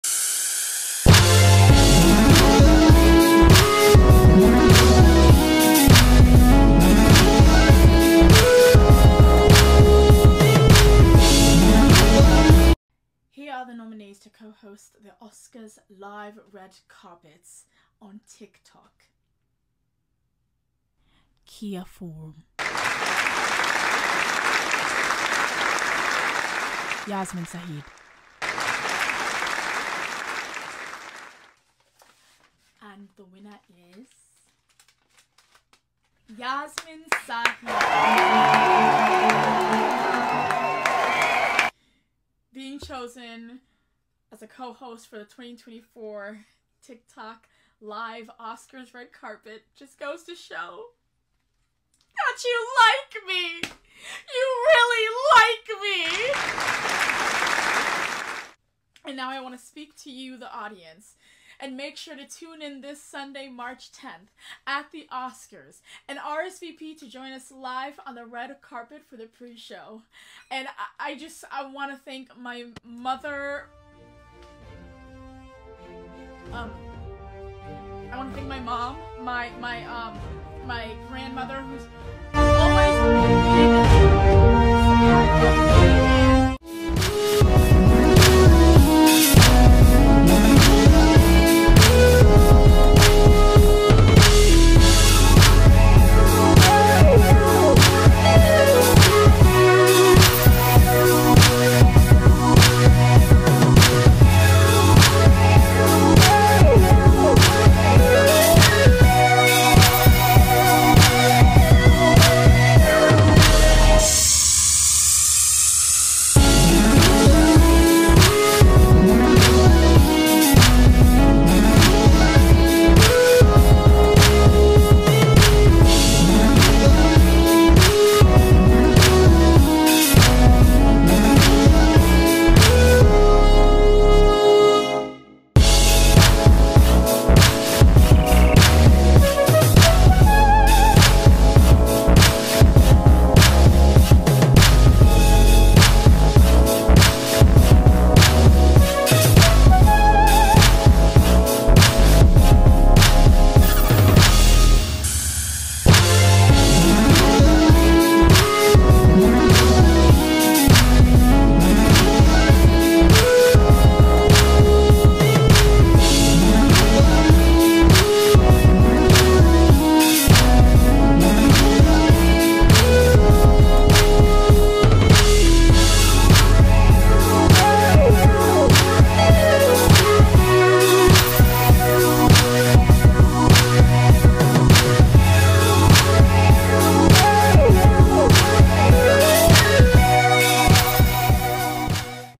I Got Invited To The Oscars! 🤯 | Oscars 2024 Red Carpet
I got invited to the Oscars! What an experience! It wouldn't have been possible without you. Thank you 💓